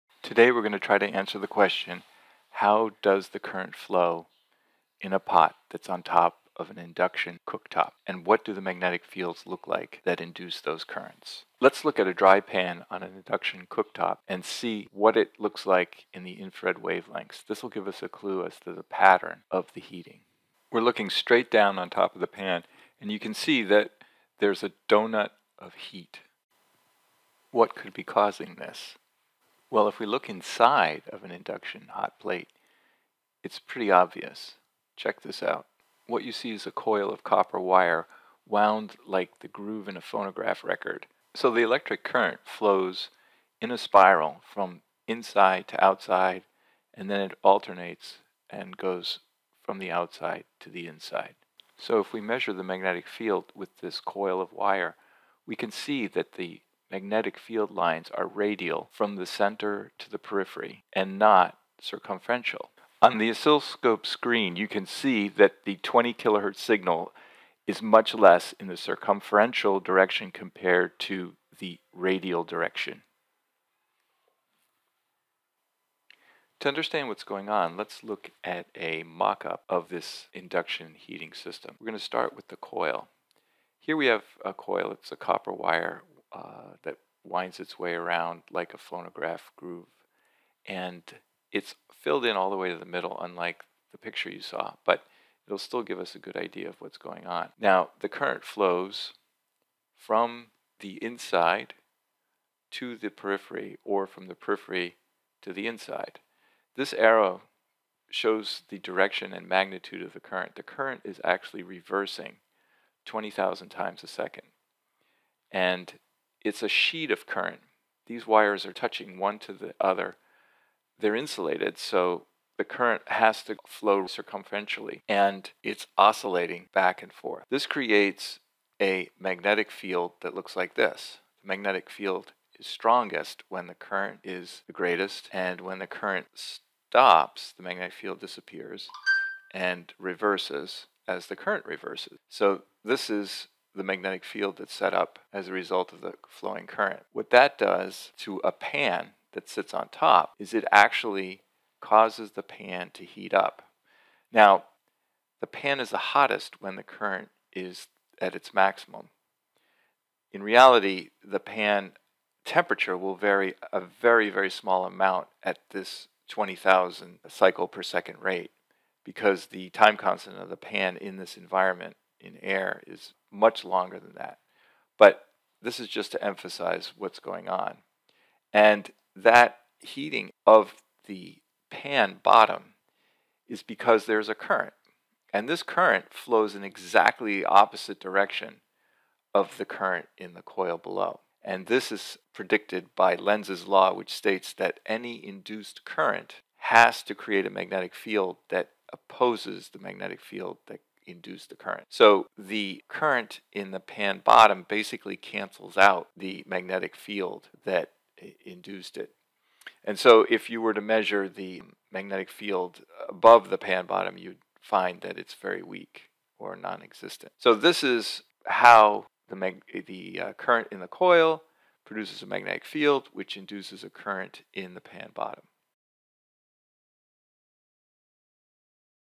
Induction Cooking Magnetic Fields and Currents
Learn how an induction cooktop uses a magnetic field to heat an induction-ready pan.

Use your scroll wheel on your mouse to zoom in and out. Click and drag to rotate. Shift click and drag to pan.